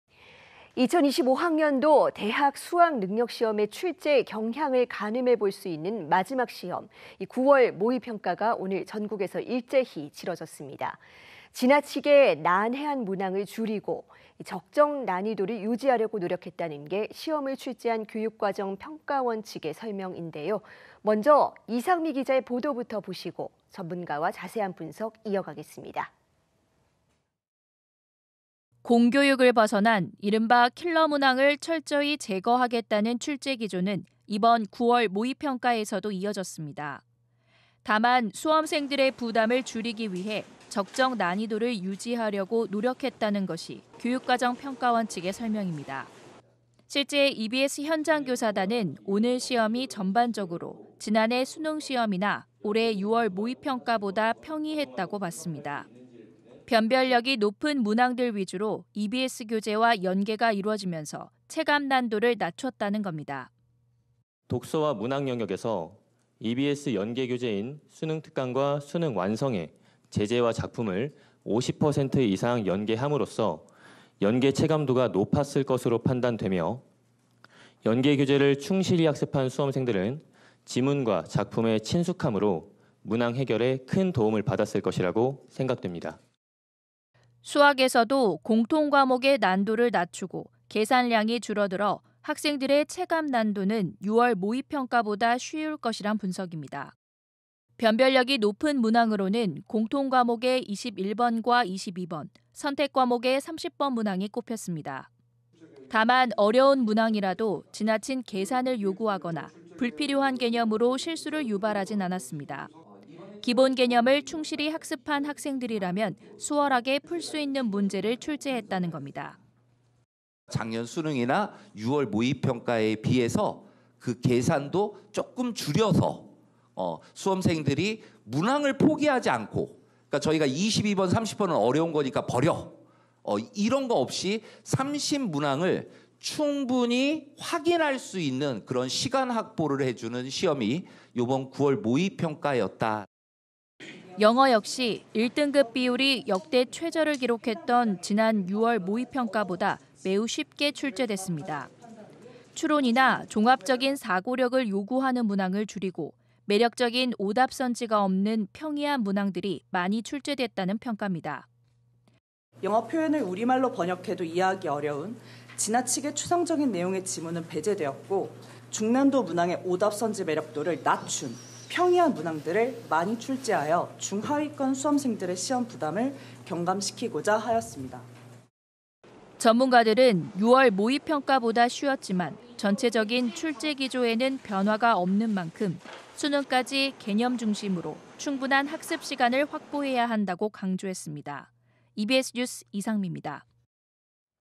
수능 '전초전' 9월 모의평가 시행…"평이한 출제" / EBS뉴스 2024. 09. 04
[EBS 뉴스]
2025학년도 대학수학능력시험의 출제 경향을 가늠해 볼 수 있는 마지막 시험인 9월 모의평가가 오늘 전국에서 일제히 치러졌습니다.

지나치게 난해한 문항을 줄이고, 적정 난이도를 유지하려고 노력했다는 게 시험을 출제한 교육과정평가원 측의 설명인데요.

먼저, 이상미 기자의 보도부터 보시고, 전문가와 자세한 분석 이어가겠습니다.

이상미 기잡니다.

[리포트]

공교육을 벗어난 이른바 킬러문항을 철저히 제거하겠다는 출제기조는 이번 9월 모의평가에서도 이어졌습니다.

다만 수험생들의 부담을 줄이기 위해 적정 난이도를 유지하려고 노력했다는 것이 교육과정평가원 측의 설명입니다.

실제 EBS현장교사단은 오늘 시험이 전반적으로 지난해 수능시험이나 올해 6월 모의평가보다 평이했다고 봤습니다.

변별력이 높은 문항들 위주로 EBS 교재와 연계가 이뤄지면서, 체감 난도를 낮췄다는 겁니다.

인터뷰: 한병훈 충남 천안중앙고 교사 / EBS 대표강사
"독서와 문학 영역에서 EBS 연계 교재인 수능 특강과 수능 완성에 제재와 작품을 50% 이상 연계함으로써 연계 체감도가 높았을 것으로 판단되며, 연계 교재를 충실히 학습한 수험생들은 지문과 작품에 친숙함으로 문항 해결에 큰 도움을 받았을 것이라고 생각됩니다."

수학에서도 공통과목의 난도를 낮추고, 계산량이 줄어들어 학생들의 체감 난도는 6월 모의평가보다 쉬울 것이란 분석입니다.

변별력이 높은 문항으로는 공통과목의 21번과 22번, 선택과목의 30번 문항이 꼽혔습니다.

다만 어려운 문항이라도 지나친 계산을 요구하거나, 불필요한 개념으로 실수를 유발하지는 않았습니다.

기본 개념을 충실히 학습한 학생들이라면, 수월하게 풀 수 있는 문제를 출제했다는 겁니다.

인터뷰: 심주석 인천 인천하늘고 교사 / EBS 대표강사
"작년 수능이나 6월 모의평가에 비해서 계산도 조금 줄여서 수험생들이 문항을 포기하지 않고 그러니까 저희가 22번, 30번은 어려운 거니까 버려, 이런 거 없이 30문항을 충분히 확인할 수 있는 그런 시간 확보를 해주는 시험이 이번 9월 모의평가였다….“

영어 역시 1등급 비율이 역대 최저를 기록했던 지난 6월 모의평가보다 매우 쉽게 출제됐습니다.

추론이나 종합적인 사고력을 요구하는 문항을 줄이고, 매력적인 오답 선지가 없는 평이한 문항들이 많이 출제됐다는 평가입니다.

인터뷰: 김예령 서울 대원외고 교사 / EBS 대표강사
"영어 표현을 우리말로 번역해도 이해하기 어려운 지나치게 추상적인 내용의 지문은 배제되었고, 중난도 문항의 오답 선지 매력도를 낮춘 평이한 문항들을 많이 출제하여 중하위권 수험생들의 시험 부담을 경감시키고자 하였습니다."

전문가들은 6월 모의평가보다 쉬웠지만, 전체적인 출제 기조에는 변화가 없는 만큼, 수능까지 개념 중심으로 충분한 학습 시간을 확보해야 한다고 강조했습니다.

EBS뉴스 이상미입니다.